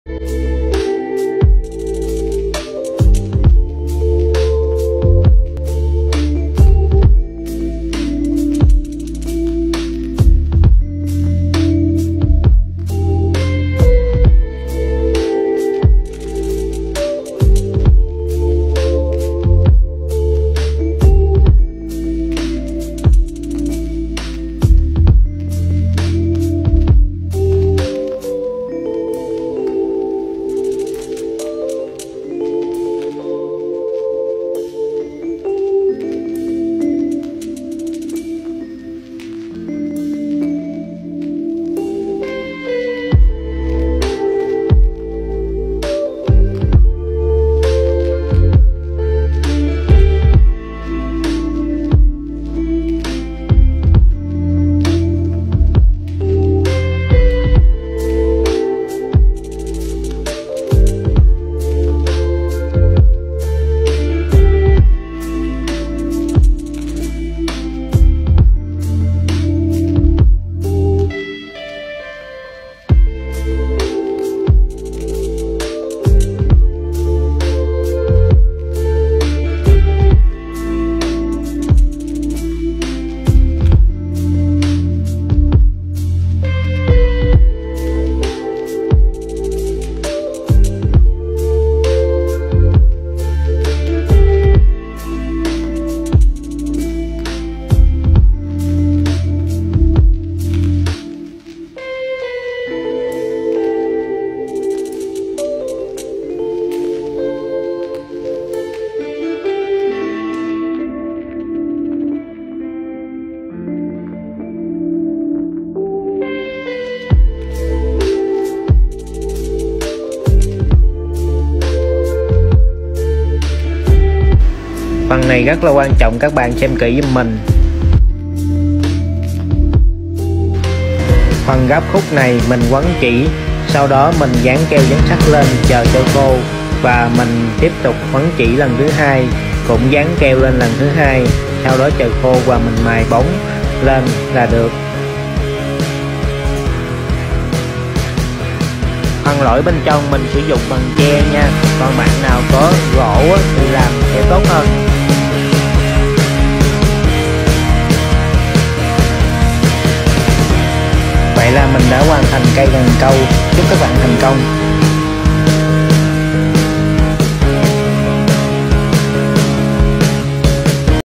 Reply @vongoc79 How to make a strong and durable bamboo fishing rod (part 2 dragon heads)😁TikTokD
copyright music @vongoc79 How to make a strong and durable bamboo fishing rod (part 2 dragon heads)😁TikTokDieuKy